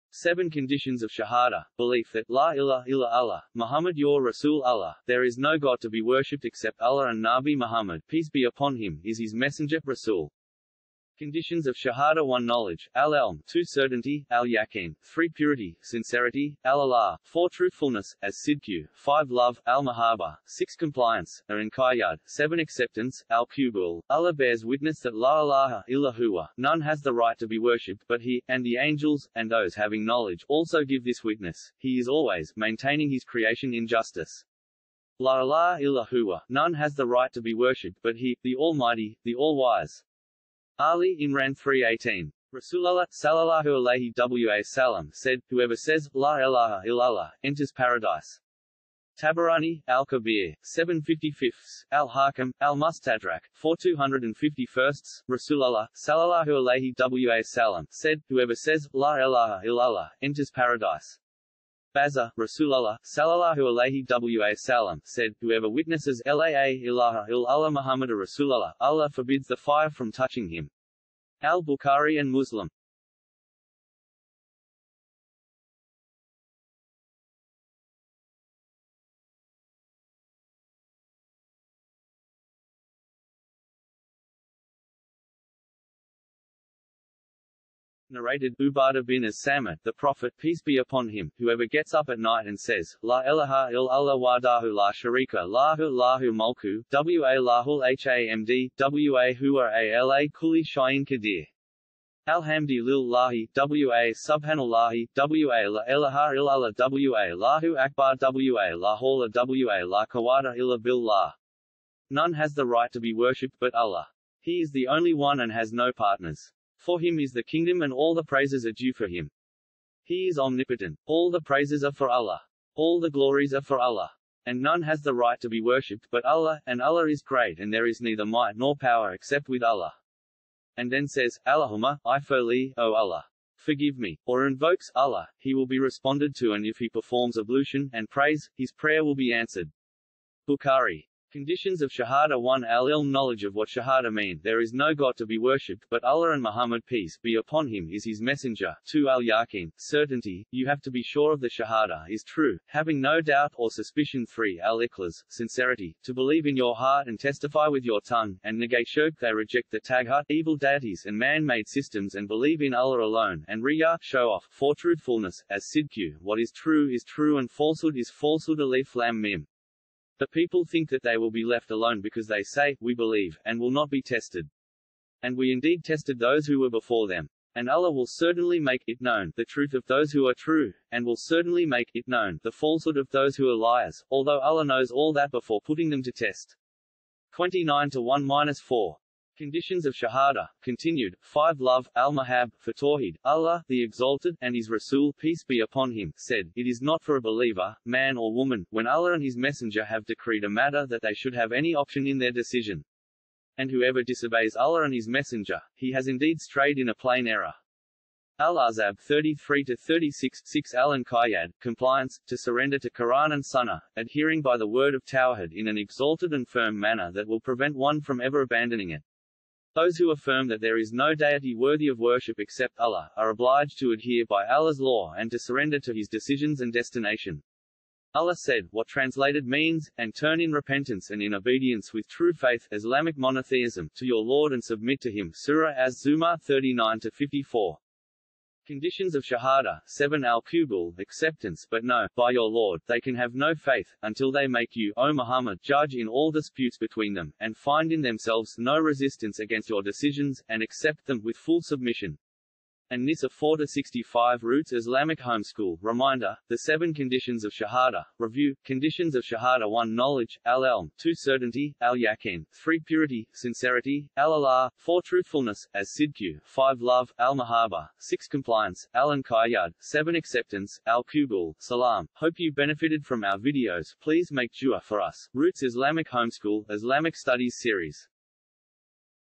7 Conditions of Shahadah
CONDITIONS OF SHAHADAH
1-
1 - Knowledge  - AL  ilm
2- Certainty -  Al Yaqeen
3- Purity/ Sincerity – Al Ikhlas
4- Truthfulness – As Sidq
5- Love – Al Mahabah
6- Compliance – A Inqiyyaad
7- Acceptance – Al Qubool ;

Rasulullah (sallallaahu alayhi wa sallam) said: "Whoever says: 'Laa ilaaha ilallaah!'

Rasulullah (sallallaahu alayhi wa sallam) said: "Whoever says: 'Laa ilaaha ilallaah!' enters Paradise." [Bazzar]

Rasulullah (sallallaahu alayhi wa sallam) said: "Whoever witnesses 'Laa ilaaha ilallaah Muhammadur Rasulullah, Allah forbids the Fire from touching him." [al-Bukhari & Muslim];

1 – Al Ilm– Knowledge of what Shahadah mean (there is no god to be worshipped but Allah and Muhammad –saw- is his Messenger);
2 – Al Yaqueen – Certainty – you have to be sure of the Shahadah is true , having no doubt or suspicion

3 – Al Iklhas – Sincerity – to believe in your Heart and Testify with your tongue – and negate Shirk they reject the Taghut (evil deities and man made systems and believe in Allah alone) and Riyaa’ (show off)

4 – Truthfullness – AsSidq – what is True is True and Falsehood is Falsehood
Alif Lam Mim. Do people think that they will be left alone because they say, We believe, and will not be tested. And We indeed tested those who were before them. And Allah will certainly make (it) known (the truth of) those who are true, and will certainly make (it) known (the falsehood of) those who are liars, (although Allah knows all that before putting them to test). [29:1-4].

5 – Love – Al Mahabb love for the Word of Taw’hid  - Loving Allah azza wa Jal – to be practicing Muslims – and abide by Allah’s Law (Shariah) – and disliking Bidah (innovations in religion);
The firmest of Iman’s ties are having loyalty in the cause of Allah, disloyalty in Allah, love in Allah and hatred for Allah’s Sake.” [At-Tabarani, Abu Dawood & Al-’Hakim]. What further testifies to the fact that love in Allah is a condition of Iman;
And of mankind are some who take (for worship) others besides Allah as rivals (to Allah). They love them as they love Allah. But those who believe, love Allah more (than anything else). [2:165].

5 – Love – Al Mahabb - for Tawheed – Allah (swa) and his Rasul (saw) (cont’d)
It is not for a believer, man or woman, when Allah and His Messenger have decreed a matter that they should have any option in their decision. And whoever disobeys Allah and His Messenger, he has indeed strayed in a plain error. [Al-Ahzab 33:36]

6 – Al Inqiyyad – Compliance - to Surrender to Qur’an and Sunna
Adhering by the Word of Taw’hid in an exalted and firm manner that will prevent one from ever abandoning it. Those who affirm that there is no deity worthy of worship except Allah, are obliged to adhere by Allah’s Law and to surrender to His decisions and destination. Allah said, what translated means, and turn in repentance and in obedience with true Faith (Islamic Monotheism) to your Lord and submit to Him [39:54];

7 – Al Qubool - Acceptance
But no, by your Lord, they can have no Faith, until they make you (O Muhammad) judge in all disputes between them, and find in themselves no resistance against your decisions, and accept (them) with full submission. [An-Nisa 4:65];